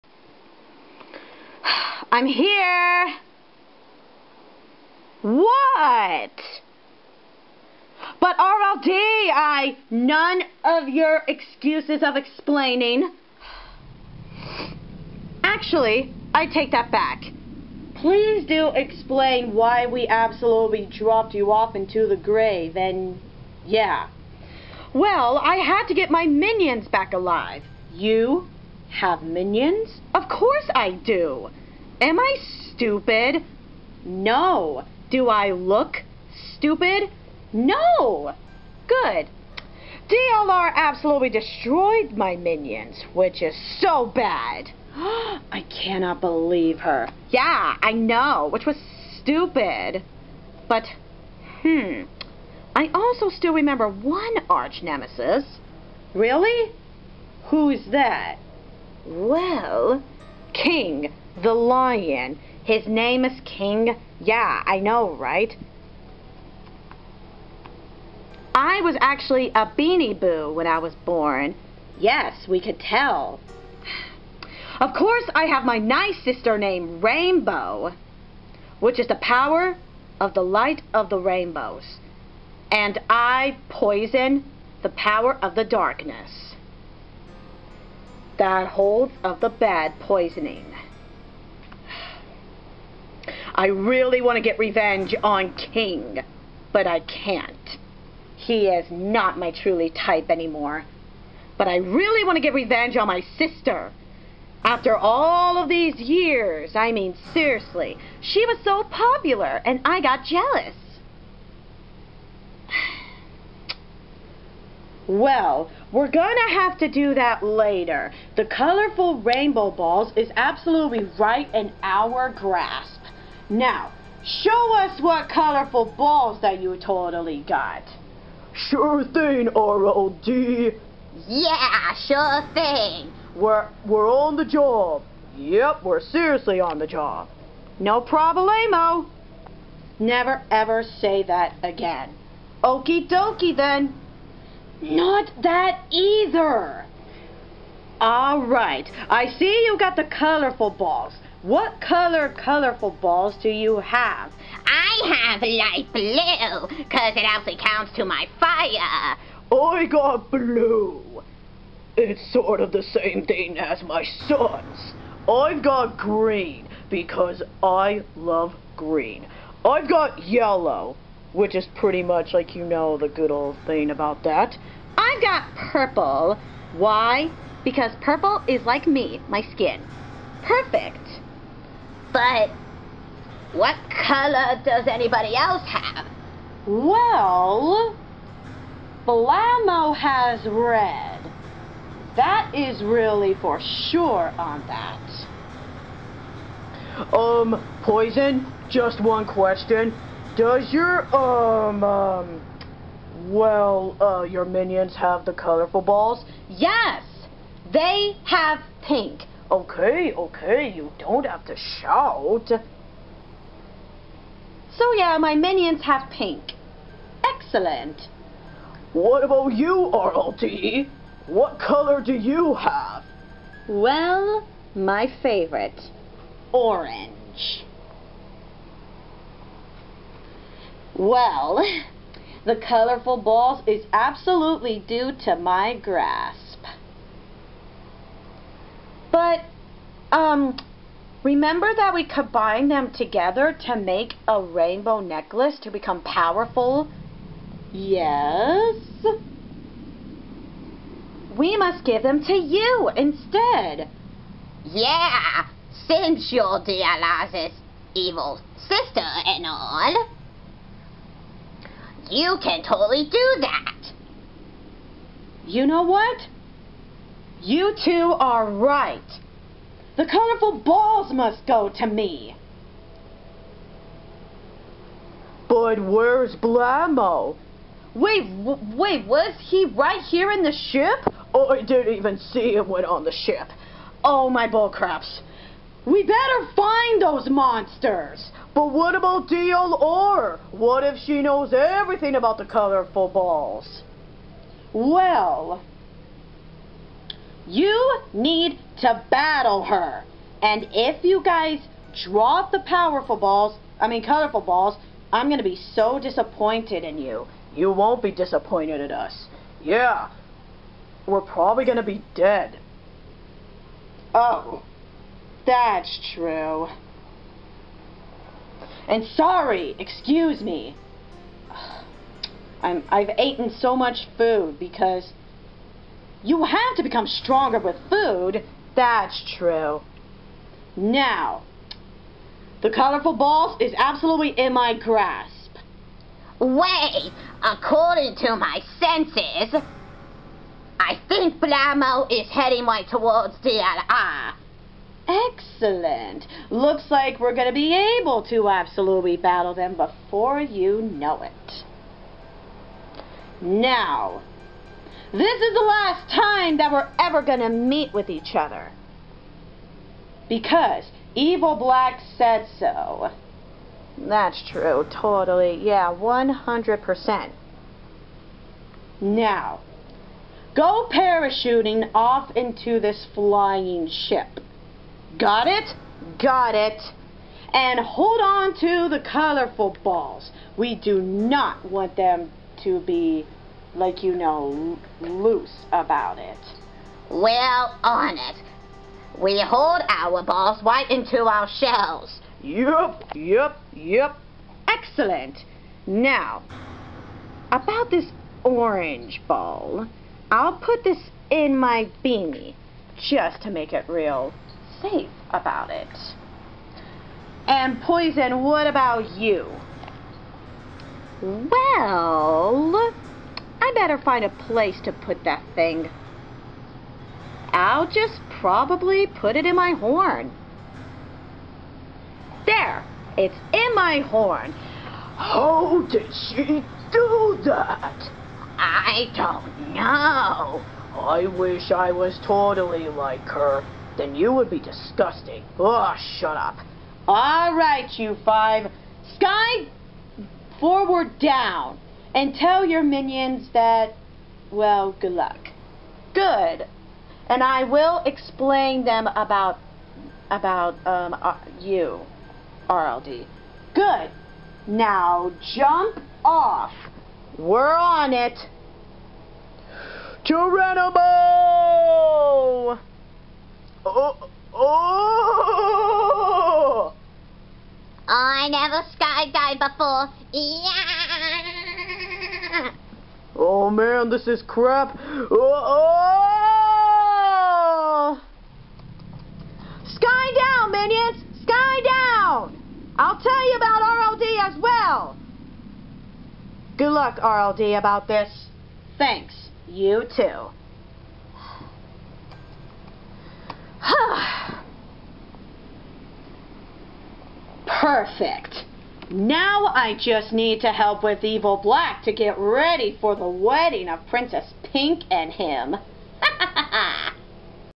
The Color Beanie Crew The Movie 20 The Finale Movie Part 5
well ladies and gentlemen the villains are now gonna get DLR and that is no good but hey DLR will get the colorful rainbow balls so let's see what happends now :)

Enjoy :D